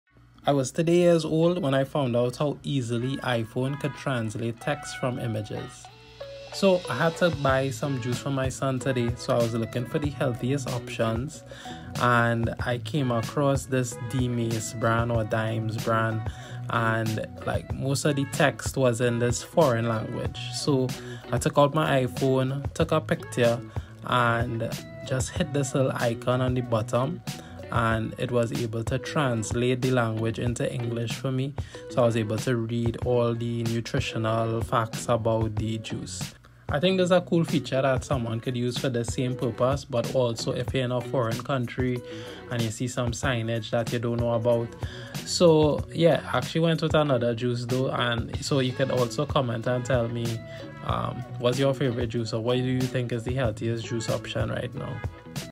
Translate languages easily with iPhone
Found out how easy it is to translate foreign languages with my iPhone when I went to buy some juice.